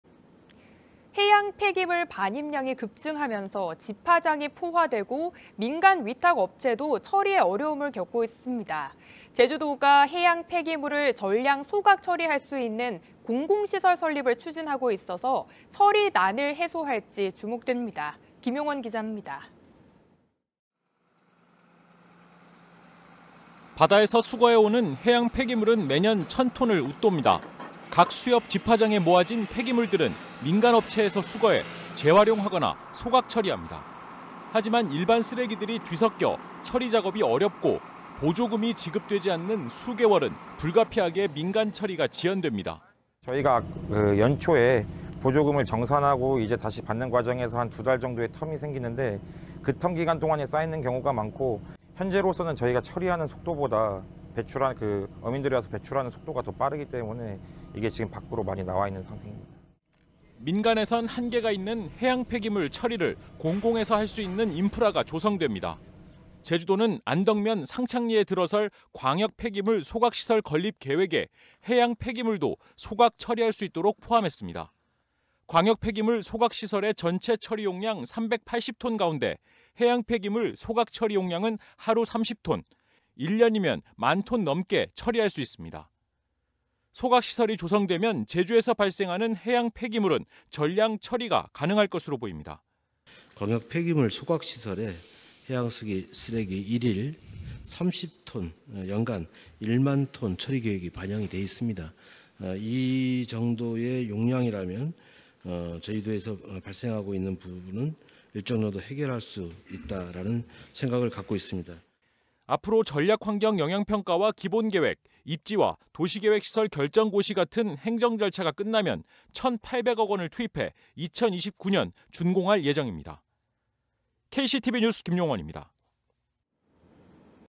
'처리난' 해양폐기물…공공처리시설 조성
해양폐기물 반입량이 급증하면서 집하장이 포화되고 민간 위탁 업체도 처리에 어려움을 겪고 있습니다.

제주도가 해양폐기물을 전량 소각 처리할 수 있는 공공시설 설립을 추진하고 있어서 처리난을 해소할 지 주목됩니다.